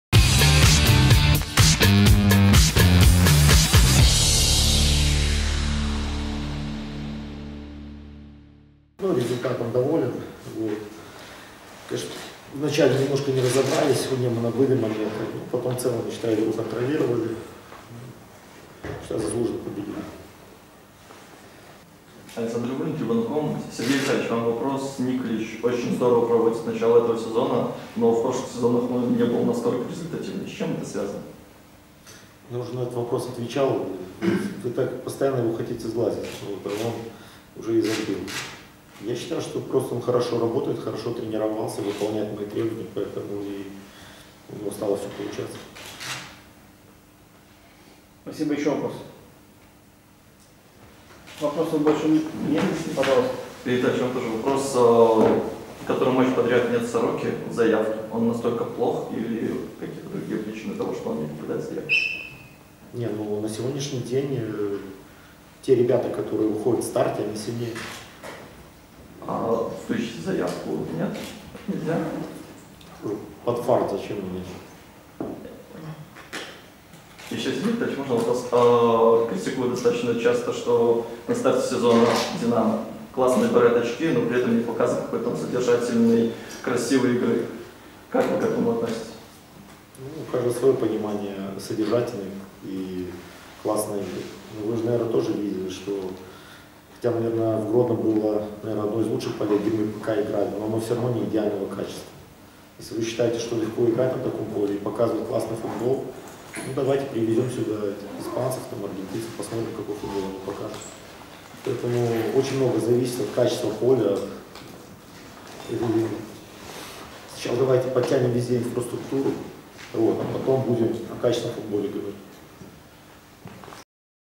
Сергей Гуренко: "Игру контролировали, заслуженно победили"
Итог матча Неман - Динамо от главного тренера.